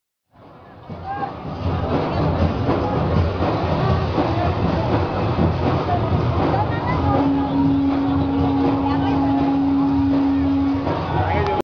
Ganesh Nimarjanam julus at Begum Bazar Hyderabad part 24
Celebration of Ganesh Nimarjanam julus at Begum Bazar Hyderabad with great enjoyment and fun from the heart